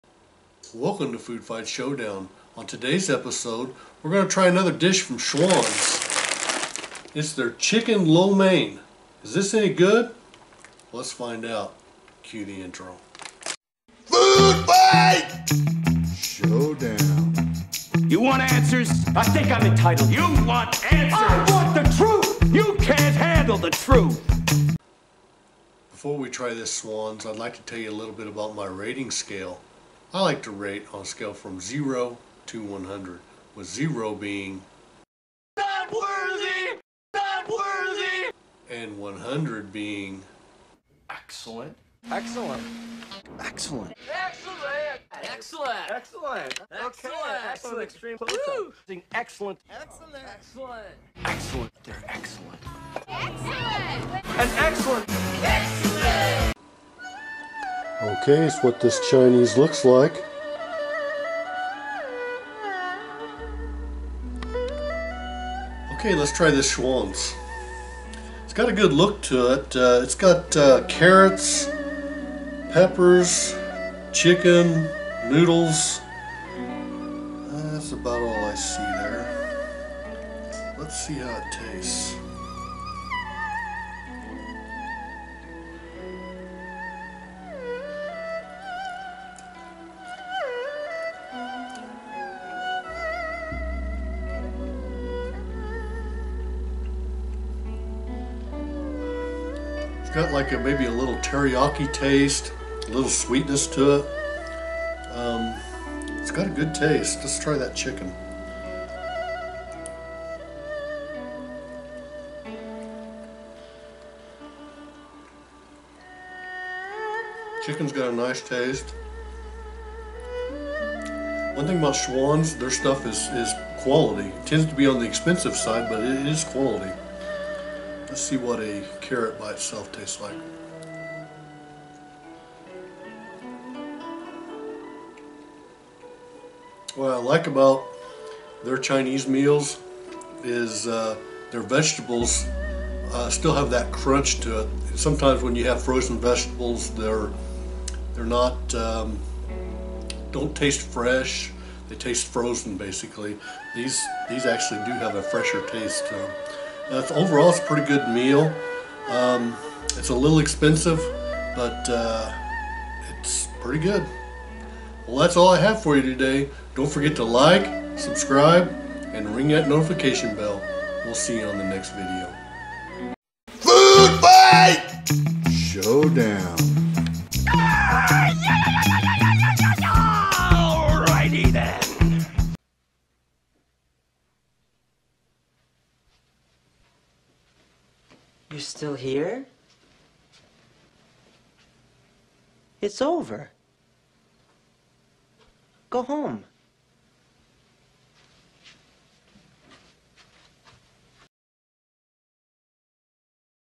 Schwan's Chicken Lo Mein
In today's video I try and review Schwan's Chicken Lo Mein.

How to contact Food Fight Showdown
                            2415 S. St. RD 59 Center point, IN 47840